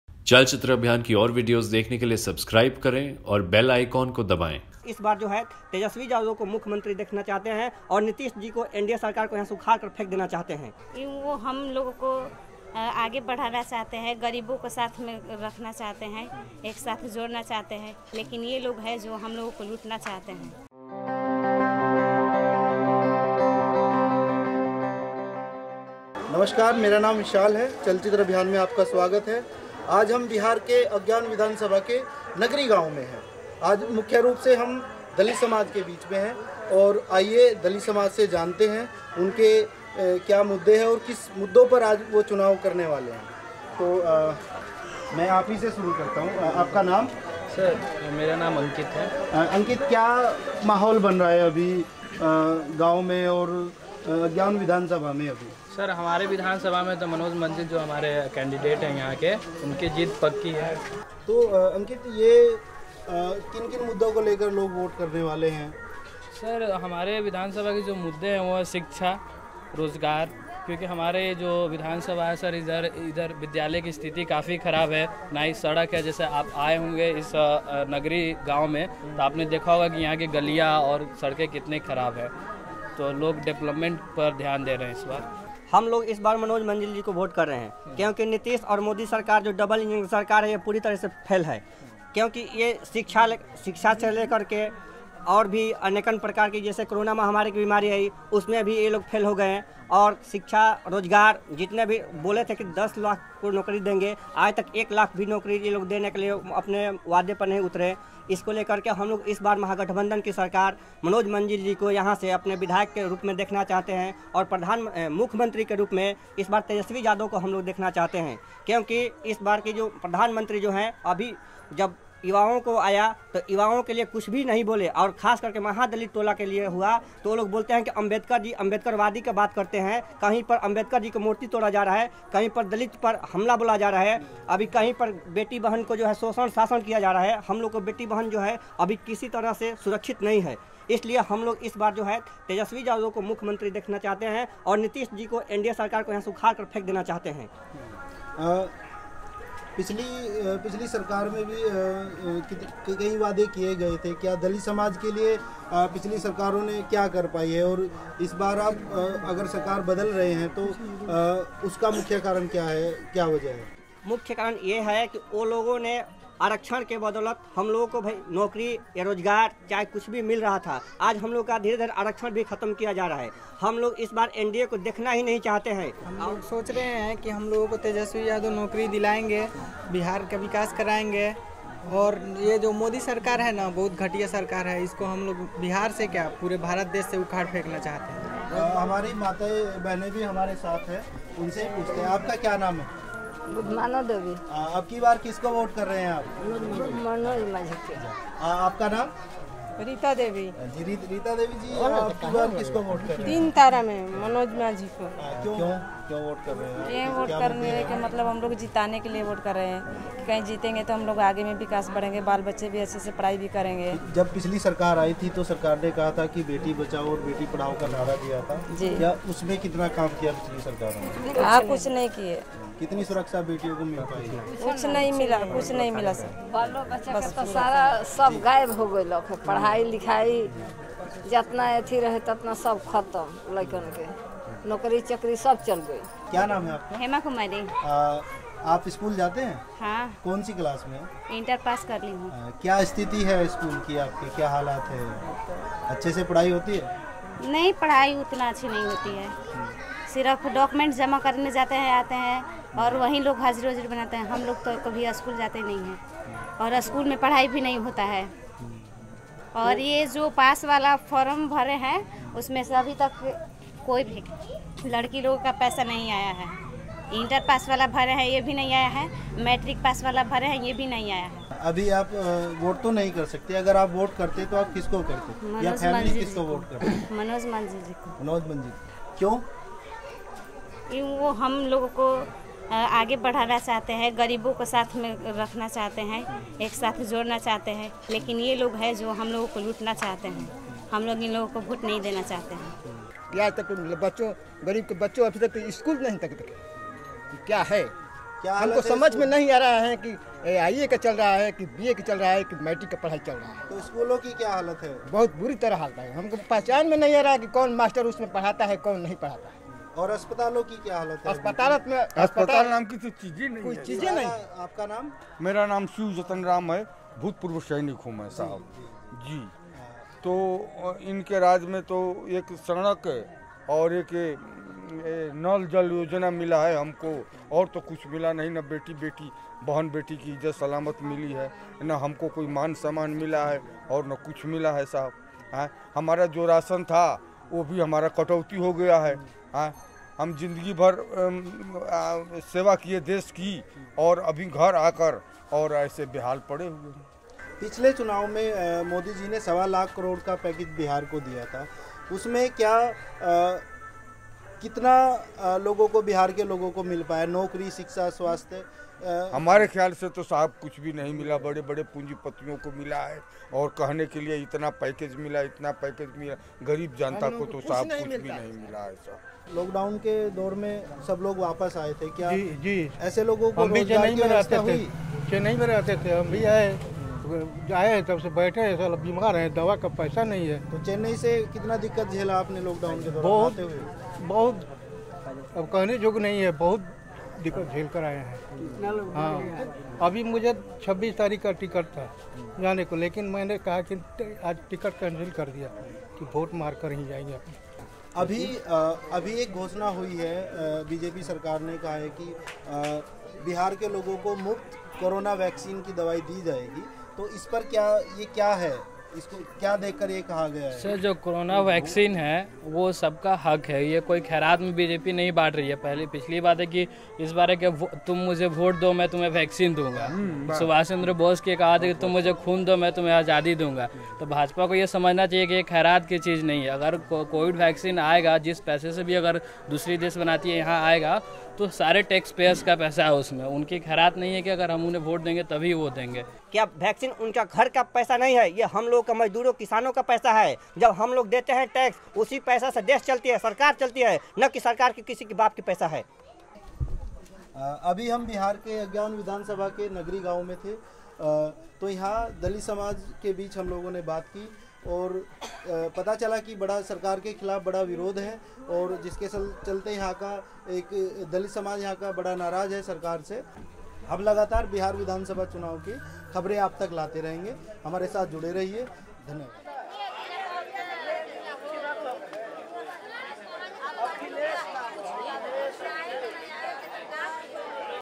बिहार : दलित क्यों NDA से नाराज़? ll Bihar: Why are Dalits angry with NDA
चलचित्र अभियान ने बिहार के भोजपुर जिले में अगिआंव विधान सभा के दलितों से बातचीत करी। हर उम्र के दलित एनडीए सरकार से नाराज़गी जताते हैं और कहते हैं कि वे मनोज मंजिल के लिए वोट करेंगे और एनडीए सरकार को हराएंगे।
टीम : विशाल स्टोनवाल , शाकिब यामीन रंगरेज़ , नकुल सिंह साव्हने, राहुल शेरवाल

ChalChitra Abhiyaan interviews Dalits from Agiaon constituency in Bhojpur district of Bihar. Dalits across ages express anger with the NDA government and they say that they will vote for Manoj Manzil and defeat the NDA government.
Team: Vishal Stonewall, Shakib Yameen Rangrez, Nakul Singh Sawhney and Rahul Sherwal